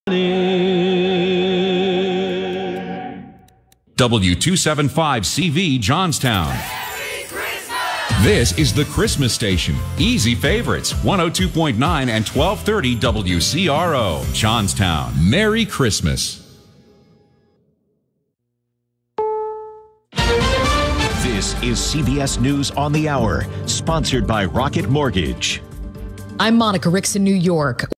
WCRO 1230 Johnstown, PA Legal ID (11/20/22)(Christmas)
Also flipped today at an unknown time

Legal identification for WCRO 1230 and translator W275CV 102.9 in Johnstown, Pennsylvania.

Owner: Greater Johnstown School District (Operated by Lightner Communications)
Format: Soft Adult Contemporary [current: Christrmas]
Branding: "Easy Favorites 102.9 & 1230 WCRO"

Recording Date: November 20th, 2022
Recording Time (Eastern): 7:00 PM